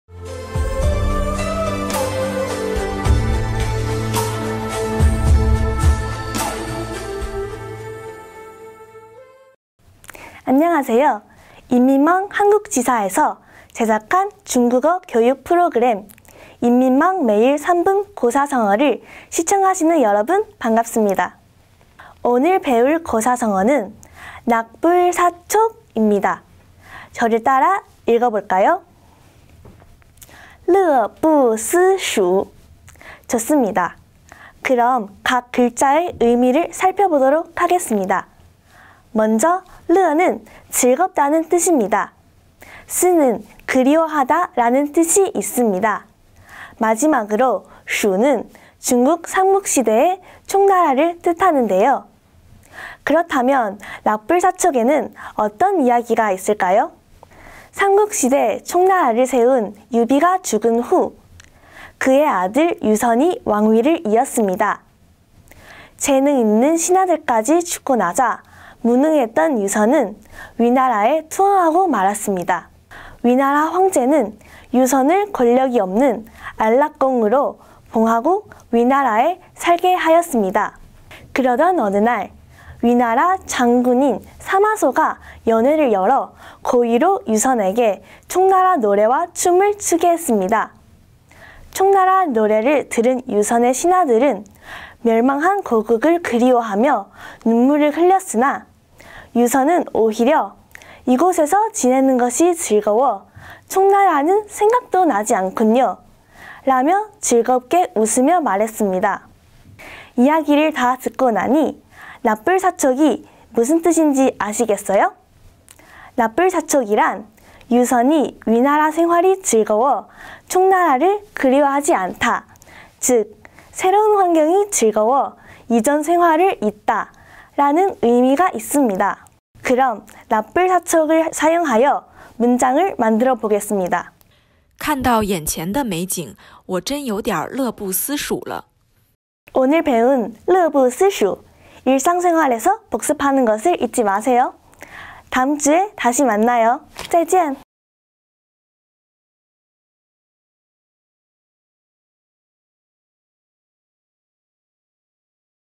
중국 고사성어 제 4화, 낙불사촉(乐不思蜀) HSK 출제 고사성어/중국어 공부/고사성어 배우기
HSK에서 자주 나오며 중국 현지에서도 흔히 사용되는 고사성어들을 옛 이야기를 통해 배우는 쉽고 유익한 중국어 강의!